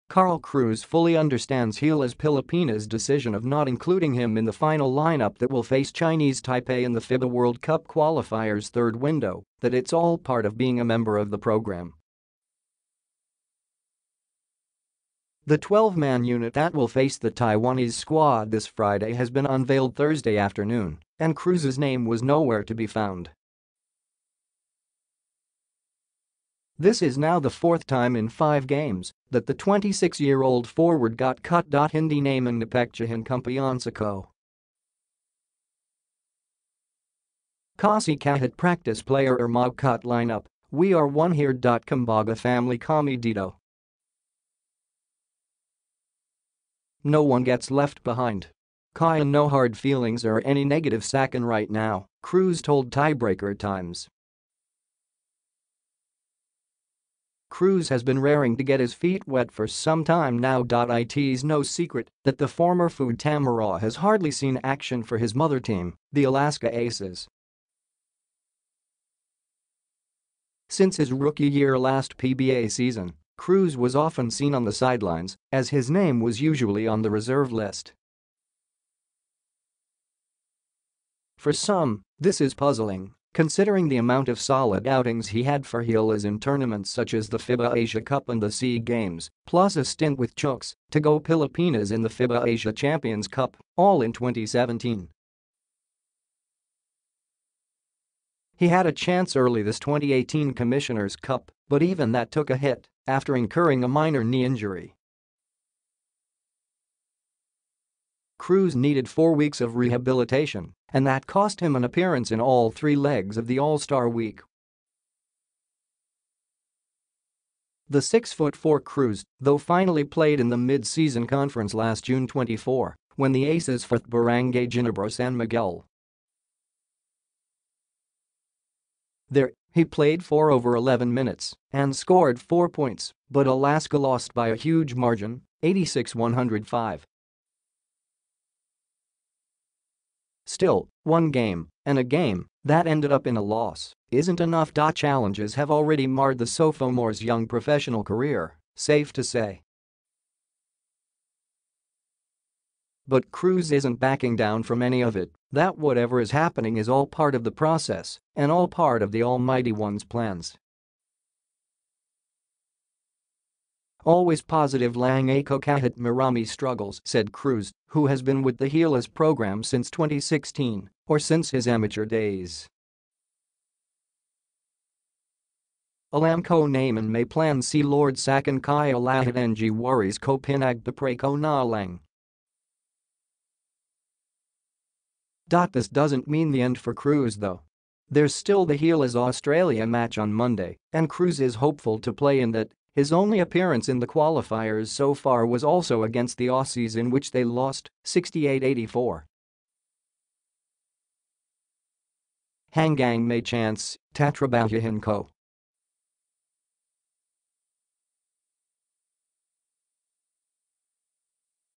No qualms whatsoever for team player Carl Cruz
Carl Cruz fully understands Gilas Pilipinas’ decision of not including him in the final lineup that will face Chinese Taipei in the FIBA World Cup Qualifiers’ third window, that it’s all part of being a member of the program.

The 12-man unit that will face the Taiwanese squad this Friday has been unveiled Thursday afternoon, and Cruz’s name was nowhere to be found. This is now the fourth time in five games that the 26-year-old forward got cut.

“Hindi naman naapektuhan kumpiyansa ko. Kasi kahit practice player or ma-cut lineup, we are one here.

“Kumbaga family kami dito. No one gets left behind. Kaya no hard feelings or any negative sa’kin right now,” Cruz told Tiebreaker Times.

Cruz has been raring to get his feet wet for some time now.

It’s no secret that the former FEU Tamaraw has hardly seen action for his mother team, the Alaska Aces. Since his rookie year last PBA season, Cruz was often seen on the sidelines, as his name was usually on the reserve list.

For some, this is puzzling, considering the amount of solid outings he had for Gilas in tournaments such as the FIBA Asia Cup and the SEA Games, plus a stint with Chooks-to-Go Pilipinas in the FIBA Asia Champions Cup — all in 2017.

He had a chance early this 2018 Commissioner’s Cup, but even that took a hit after incurring a minor knee injury. Cruz needed four weeks of rehabilitation, and that cost him an appearance in all three legs of the All-Star Week.

The 6-foot-4 Cruz though finally played in the mid-season conference last June 24, when the Aces fought Barangay Ginebra San Miguel. There, he played for over 11 minutes and scored four points, but Alaska lost by a huge margin, 86-105.

Still, one game — and a game that ended up in a loss — isn’t enough.

Challenges have already marred the sophomore’s young professional career, safe to say. But Cruz isn’t backing down from any of it, that whatever is happening is all part of the process and all part of the Almighty One’s plans.

“Always positive lang ako kahit marami struggles,” said Cruz, who has been with the Gilas program since 2016, or since his amateur days.

“Alam ko naman may plan si Lord sa’kin kaya lahat ng worries ko pinagpepray ko na lang.”

This doesn’t mean the end for Cruz though. There’s still the Gilas-Australia match on Monday, and Cruz is hopeful to play in that — his only appearance in the Qualifiers so far was also against the Aussies in which they lost, 68-84.

“Hanggang may chance, tatrabahuhin ko.”